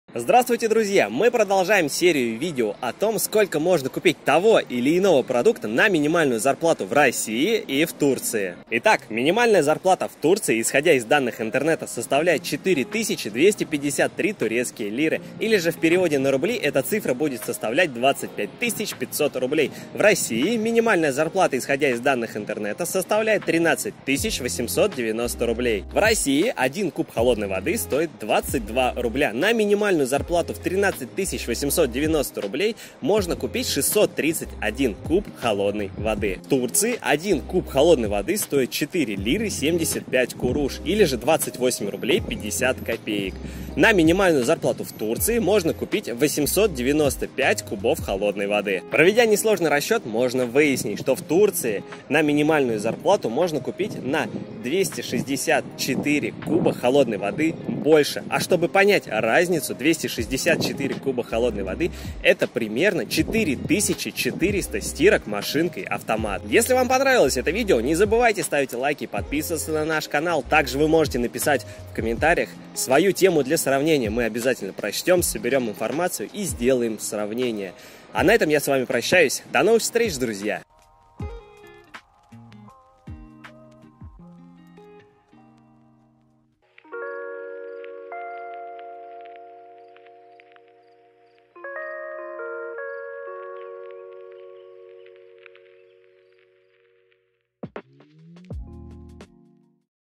РЕАЛЬНАЯ ЖИЗНЬ В ТУРЦИИ. Сколько Можно Купить Того Или Иного Продукта В РОССИИ И ТУРЦИИ
Сравним, сколько же можно купить конкретного продукта в России и в Турции на минимальную зарплату. Уверен,будет очень интересно и познавательно, так что:
Присаживайтесь по удобнее, мы начинаем👌